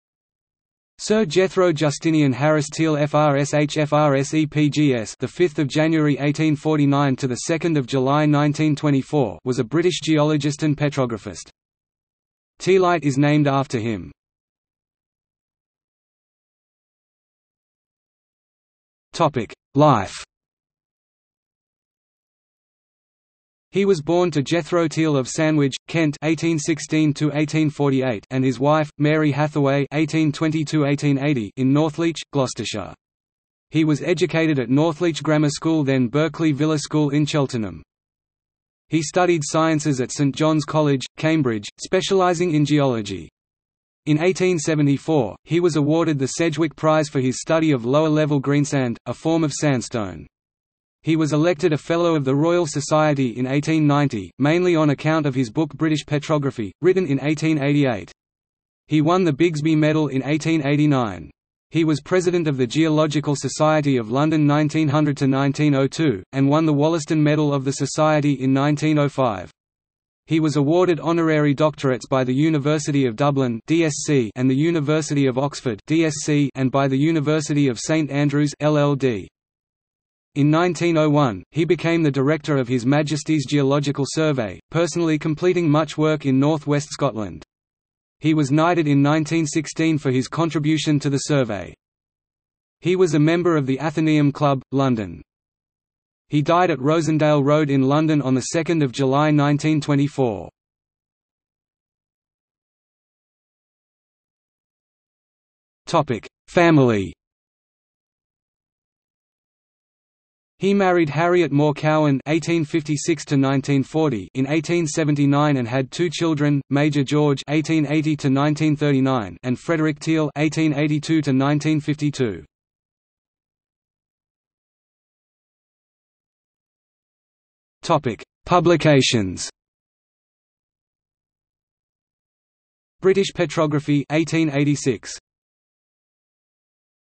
Jethro Teall | Wikipedia audio article
00:00:15 1 Life
00:01:42 2 Family
00:02:04 3 Publications

- Socrates

SUMMARY
Sir Jethro Justinian Harris Teall FRS HFRSE PGS (5 January 1849 – 2 July 1924) was a British geologist and petrographist.
Teallite is named after him.